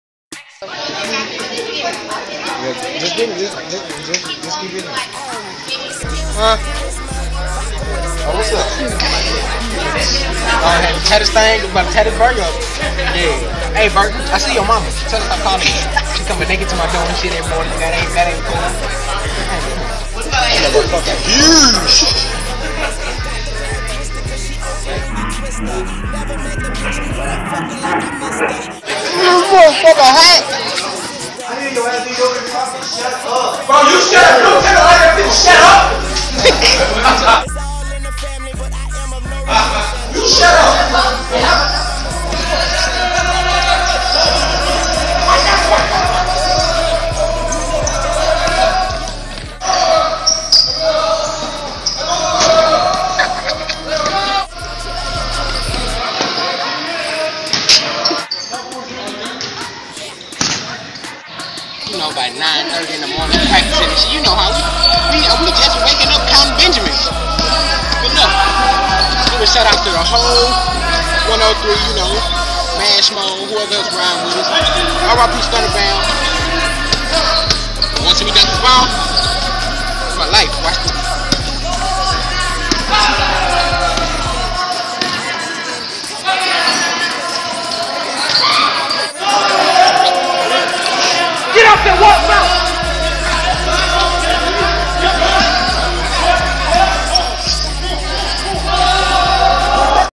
Shannon Freeman's Every Day Life
Shannon Freeman messing around at WaterBurger, the school practice, and the Locker Room.